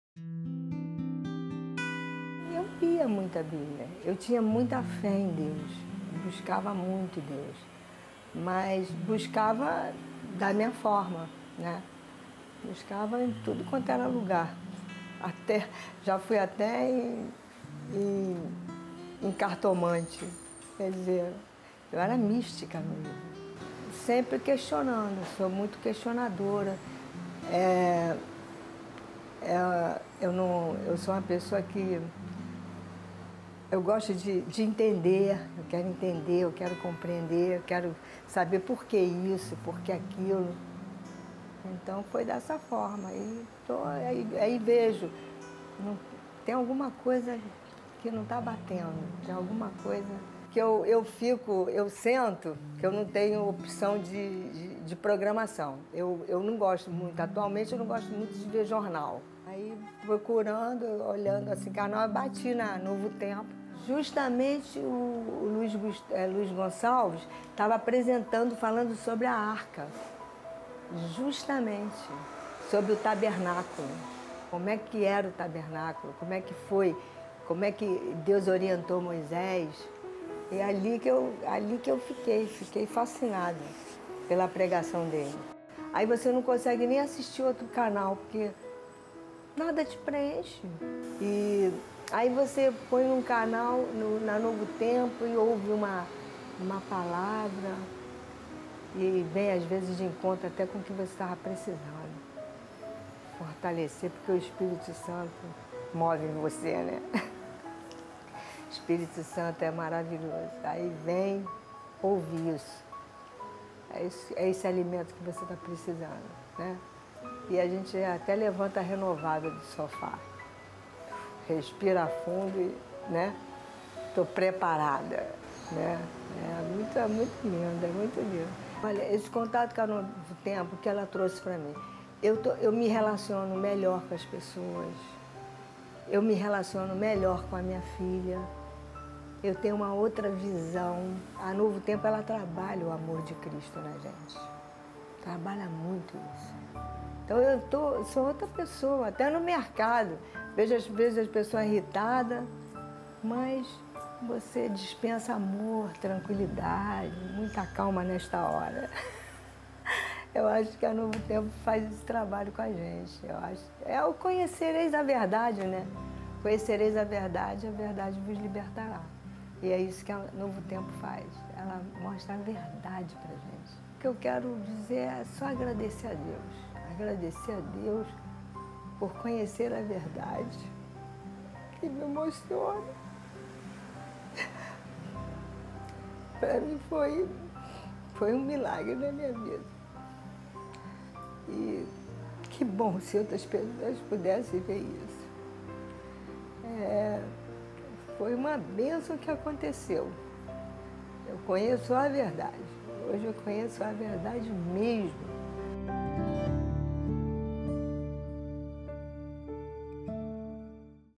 [TESTEMUNHO] Neusa (2020)
Neusa tinha dúvidas sobre a Bíblia, mas não sabia como responde-las. Certo dia, enquanto zapeava sua televisão, ela viu o pastor Luis Gonçalves falando. E a mensagem dela era diretamente para ela! Neusa ficou muito impressionada e nunca mais largou a Novo Tempo. Veja aqui um pouco mais sobre a história dela!